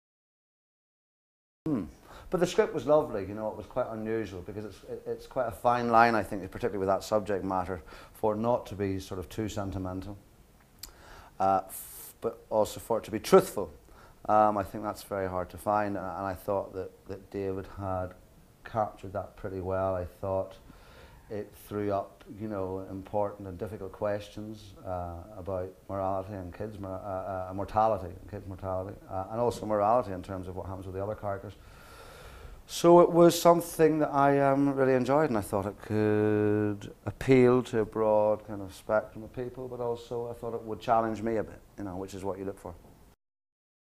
Matching Jack :: Interview with James Nesbitt
Cascade Films presents Matching Jack, a film from David Parker and Nadia Tass.